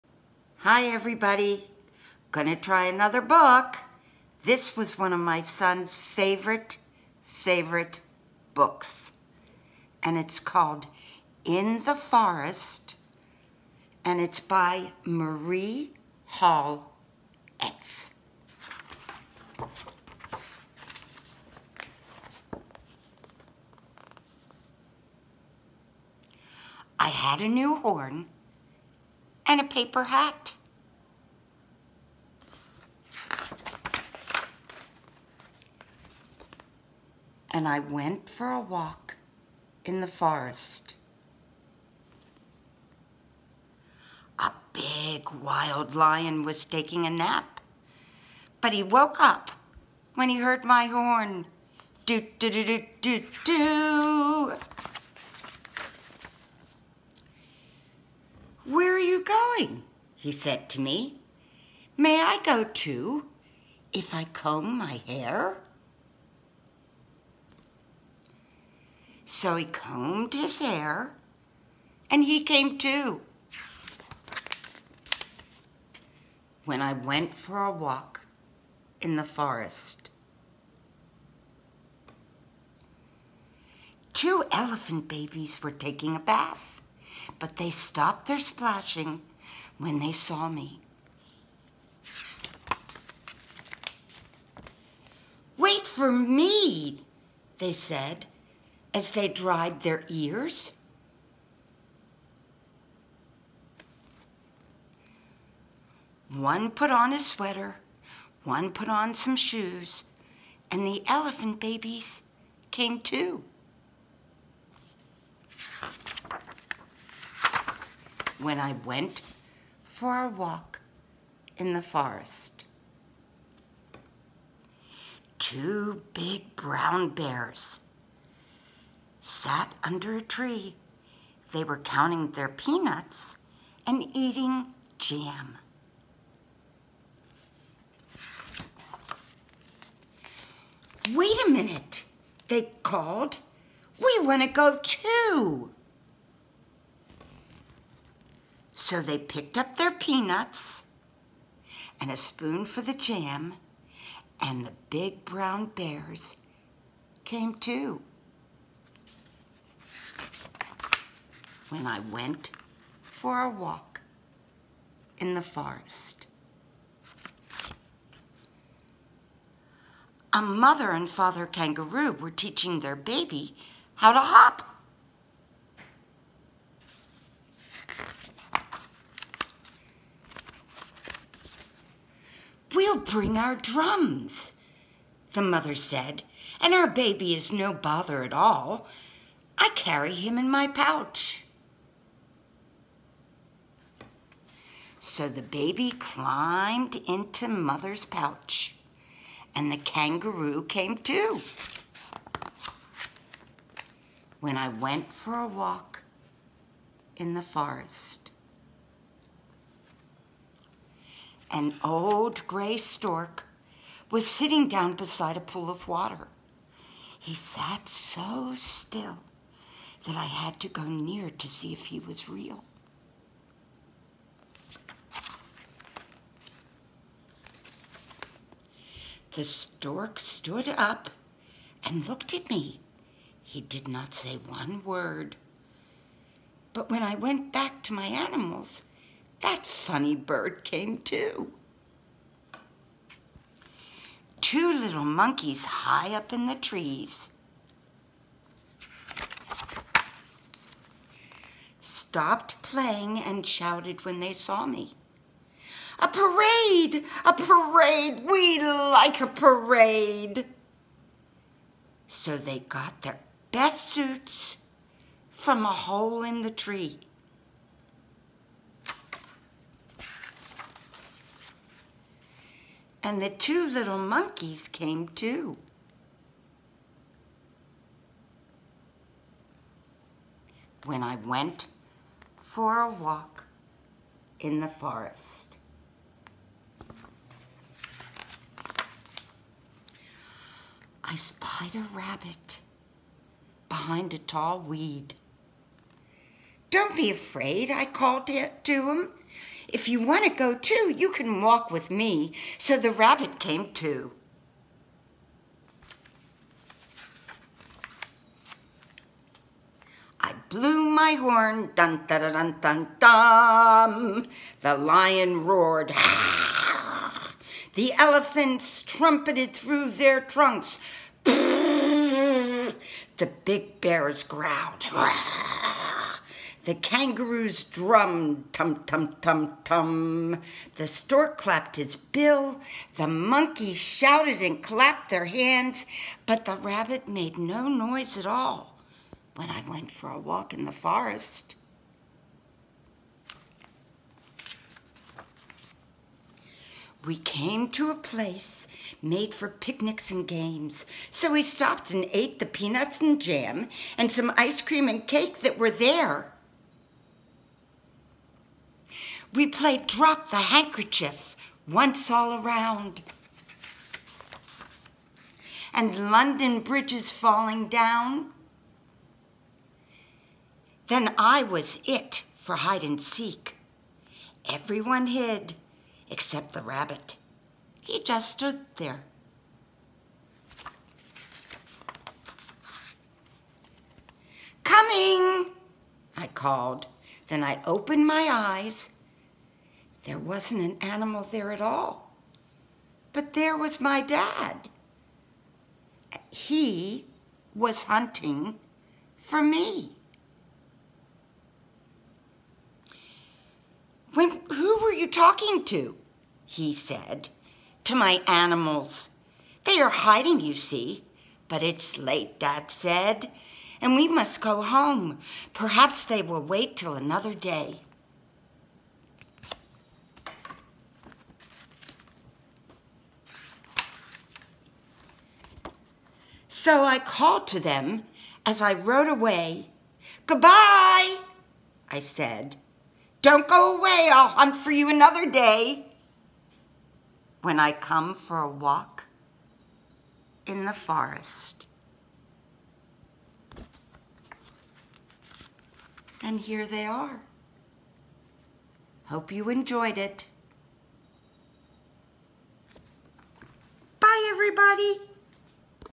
In The Forest by Marie Hall Ets
Reading to preshoolers